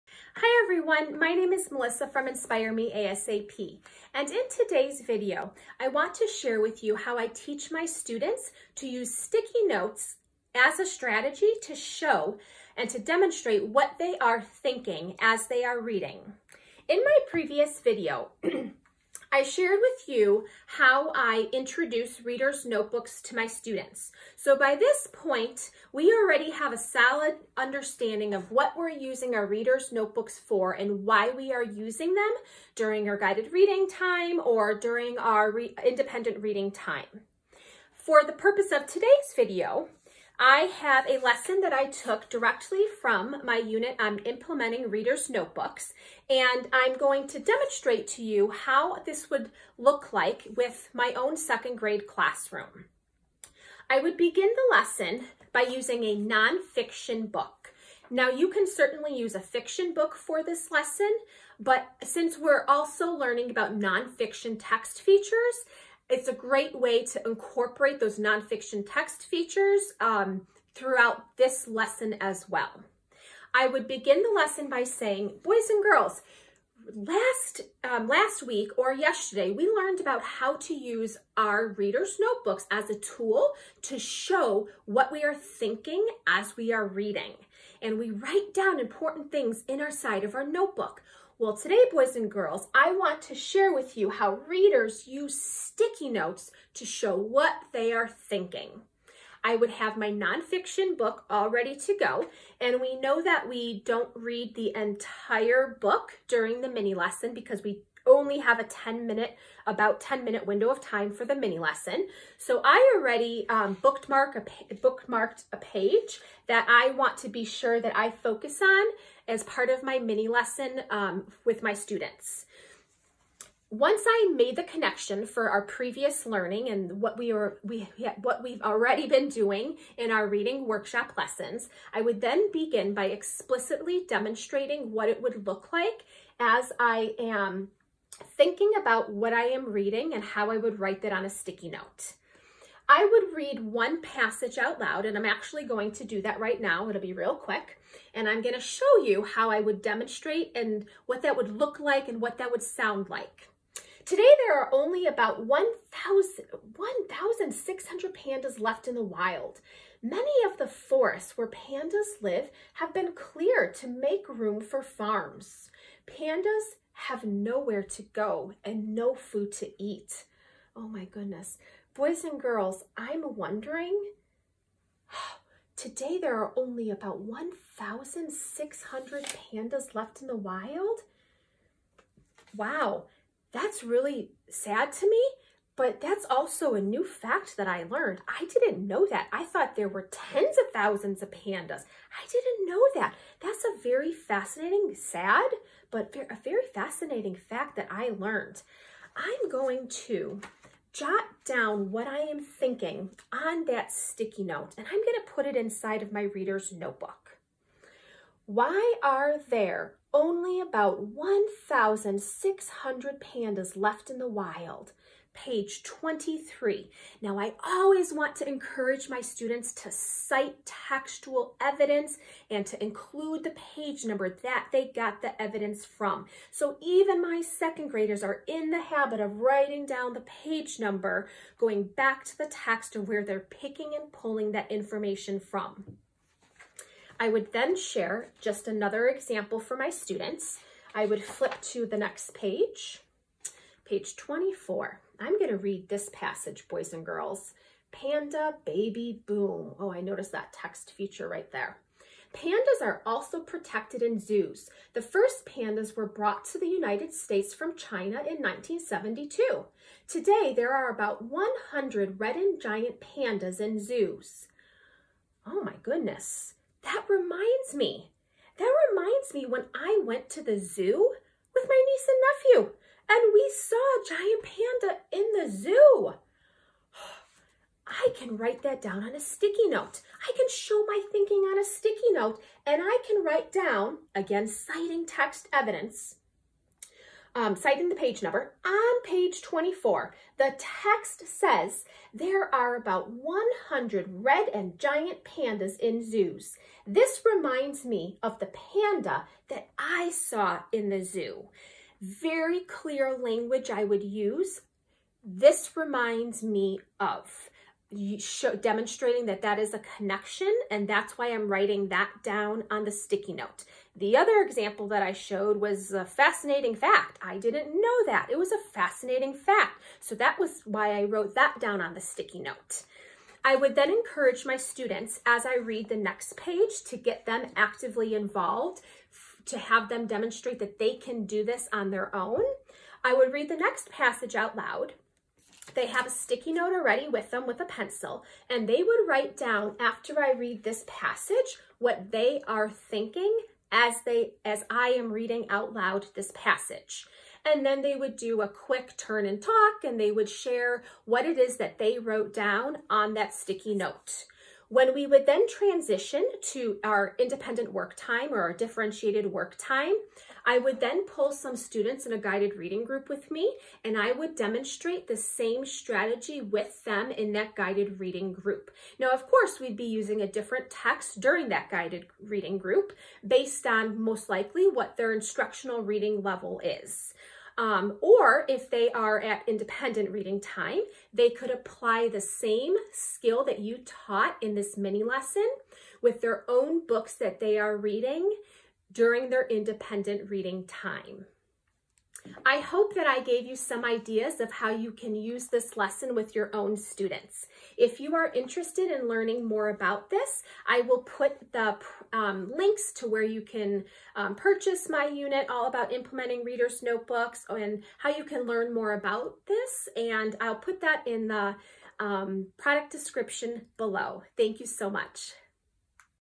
Stop and Jot Reading Strategy
In this video, I share with you how I teach my students to use sticky notes as a way to show what they are thinking as they are reading. This reading strategy is called stop and jot and is an important way for students to self-monitor what they are reading. When we ask students to write about what they are reading, we can clearly see their level of comprehension. In my own classroom, I have used successfully used reader's notebooks with my own second and third graders, as a way for them to express their thinking about what they are reading. I created this short video to show you how I teach my students to use sticky notes for their reader's notebooks.

I am passionate about helping teachers implement reader's workshop in their primary classrooms!

Happy Reading!
Melissa Kozerski